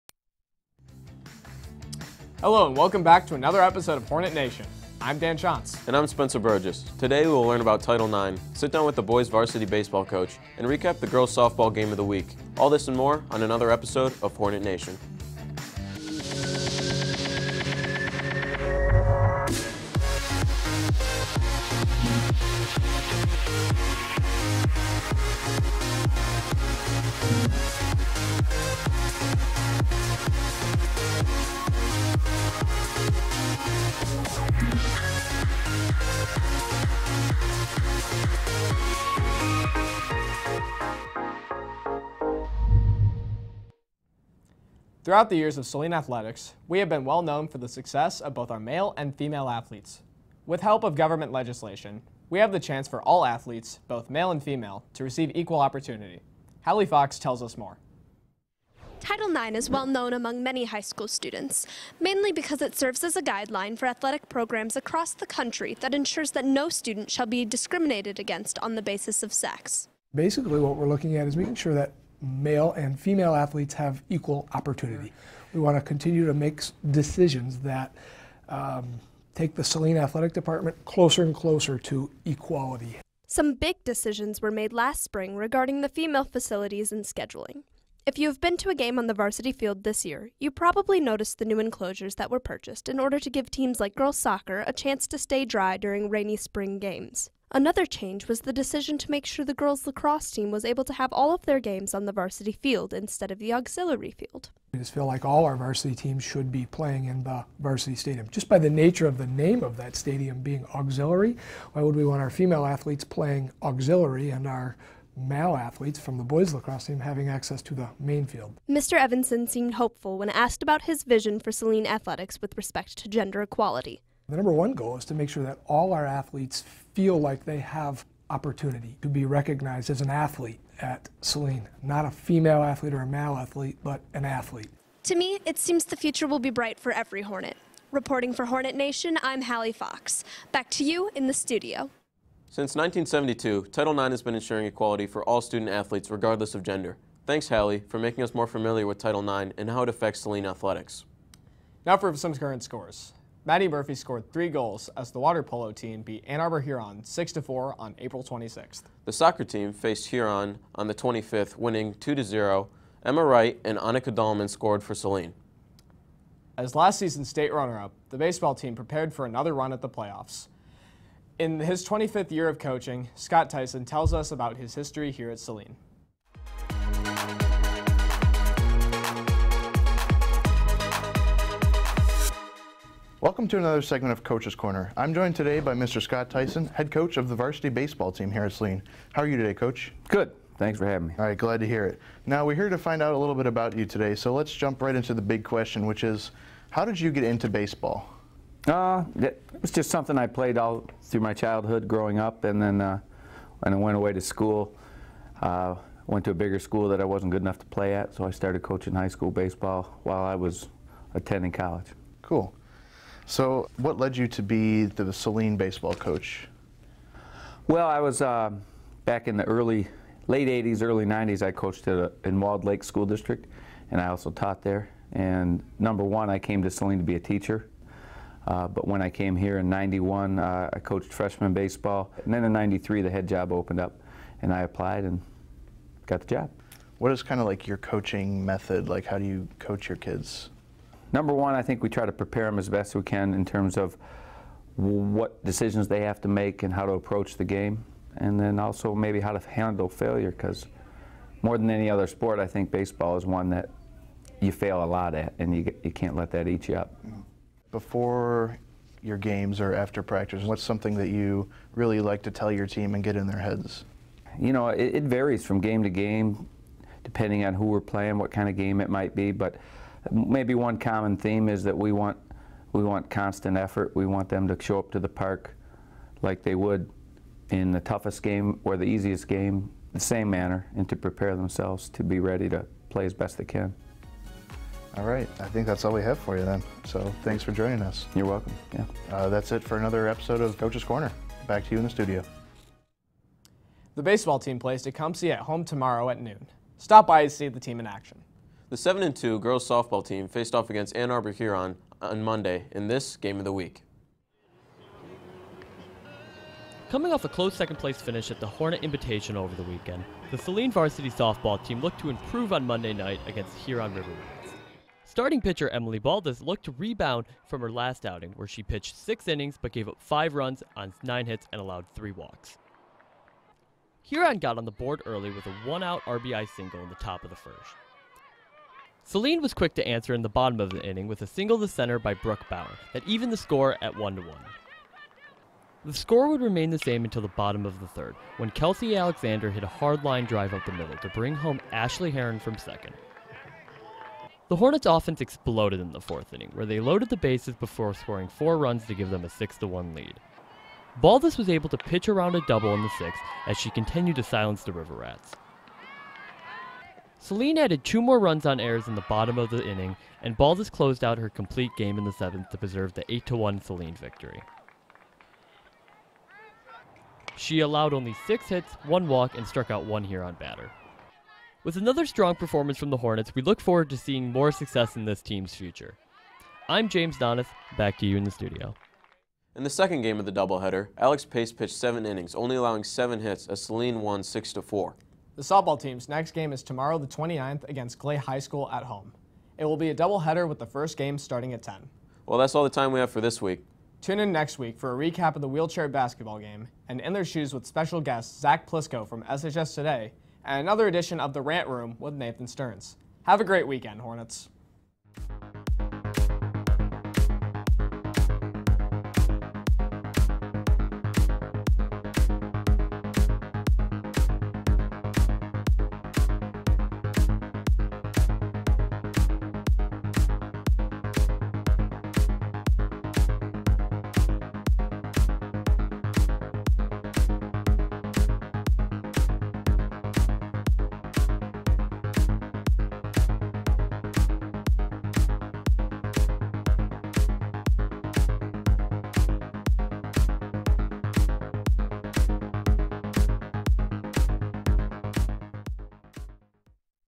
HN 4.28.17
Hornet Nation - April 28, 2017

Hosts:
Dan Schantz
Spencer Burgess

Features:
Title Nine
Game of the Week: Softball
Coaches Corner with Scot Theisen